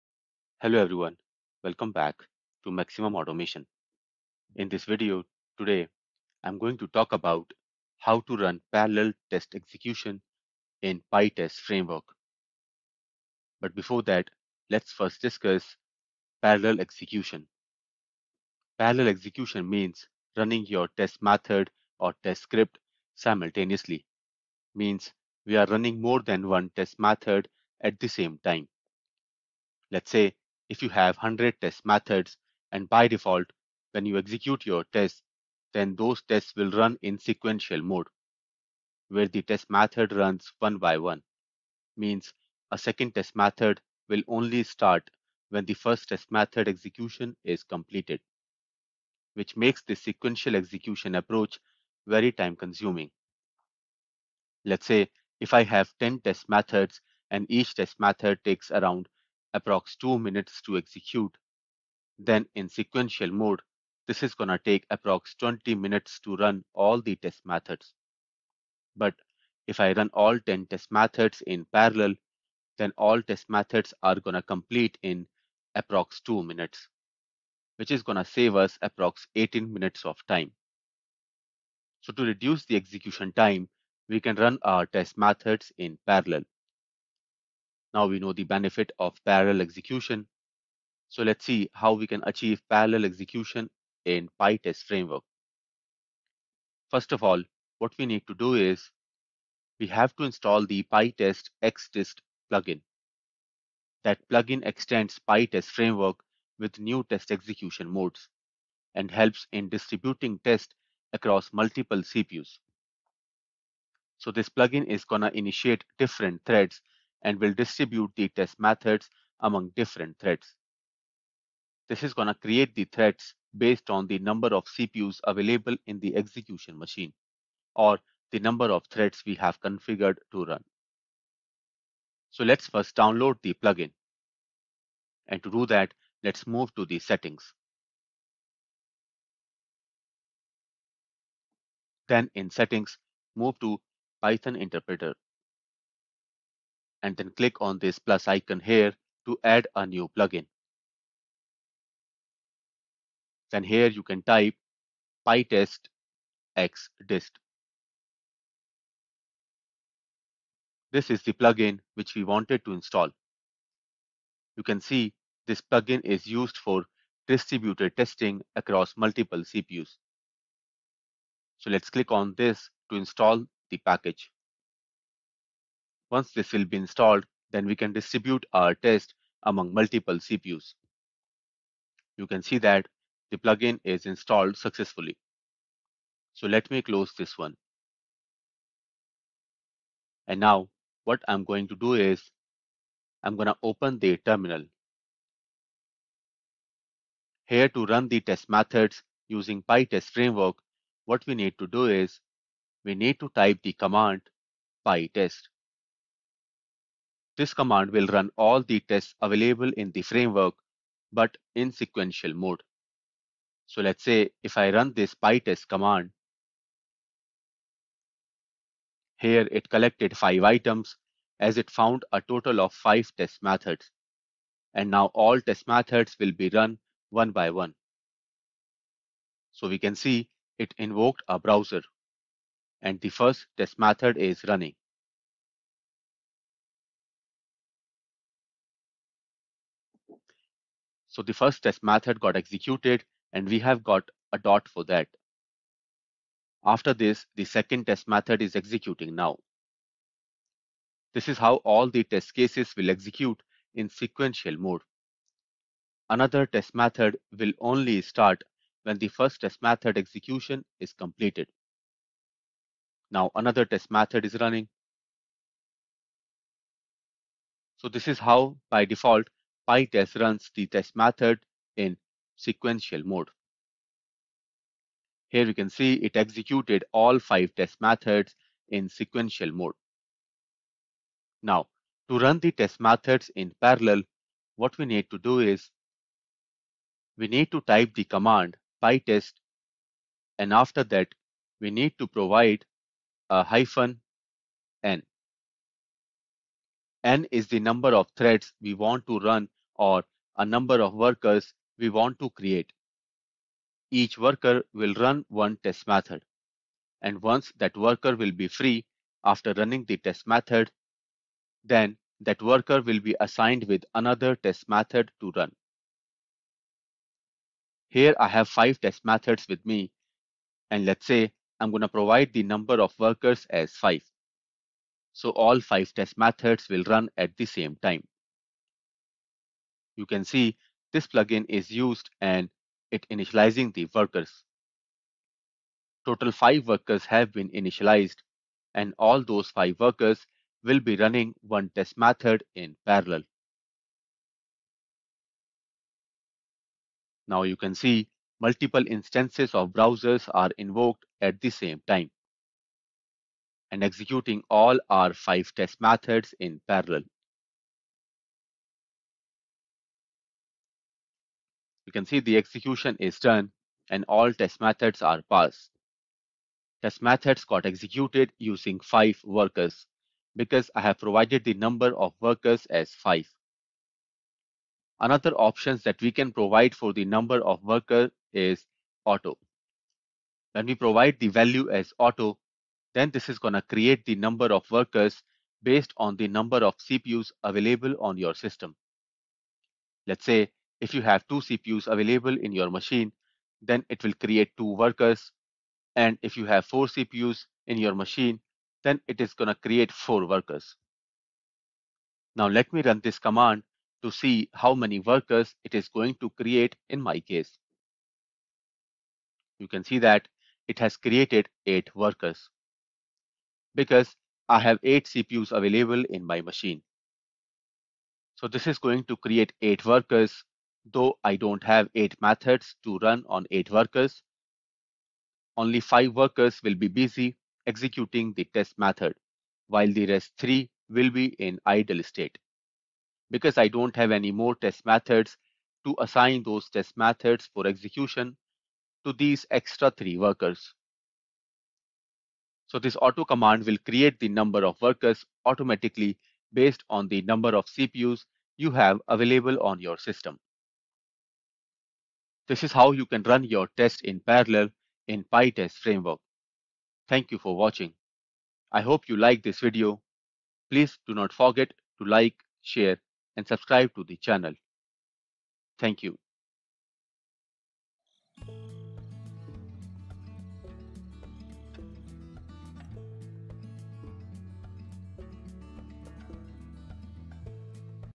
Part 13 | Selenium With Python Tutorial For Beginners | Parallel Execution in Pytest Framework
⚡ Selenium With Python Tutorial For Beginners | Parallel Test Execution in Pytest Framework

Want to speed up your Selenium Python test suite? In this tutorial, learn how to run your tests in parallel using the powerful pytest-xdist plugin! Parallel execution helps reduce your test run time significantly, making your automation more efficient and scalable.

🚀 What You’ll Learn:
Why parallel test execution matters in automation

How to install and configure the pytest-xdist plugin

Running Selenium tests in parallel with pytest

Best practices to avoid common pitfalls with parallel execution

Tips on managing test data and browser sessions for parallel runs

🧰 Tools & Libraries Used:
Selenium WebDriver (Python)

Pytest framework

pytest-xdist plugin for parallel execution